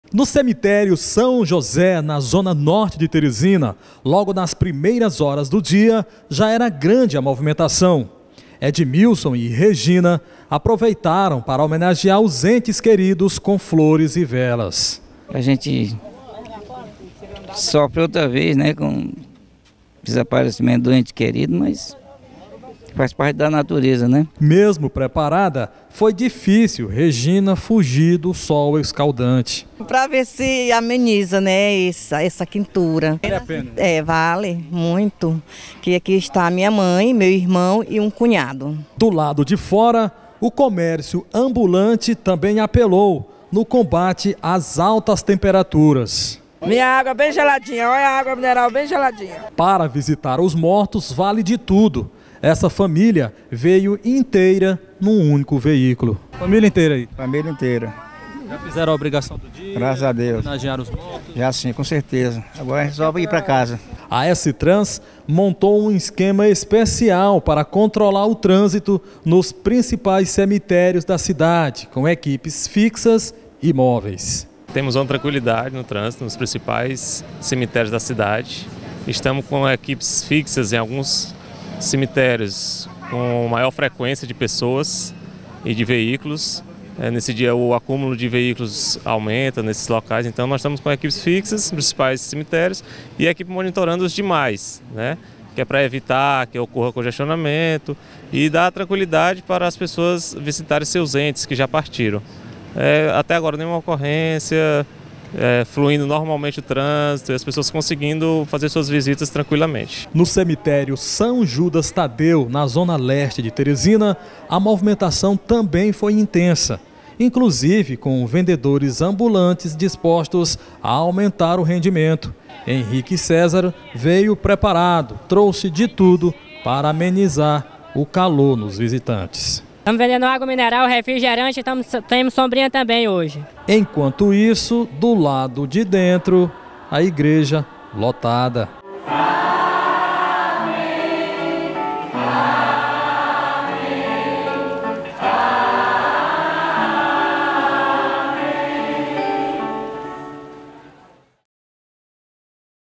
Movimento nos cemitérios de Teresina é intenso no Dia de Finados
Mais de 10 mil pessoas visitaram os cemitérios da capital no Dia de Finados, uma demonstração de saudade e momento de ficar mais perto de lembranças de parentes e amigos.